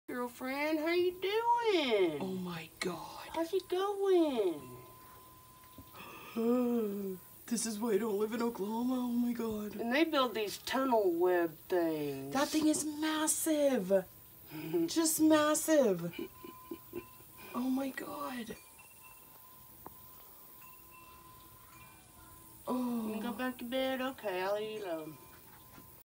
Rush Springs Oklahoma
My stepmother Elizabeth calls this “the nightmare spider. I will definitely be having nightmares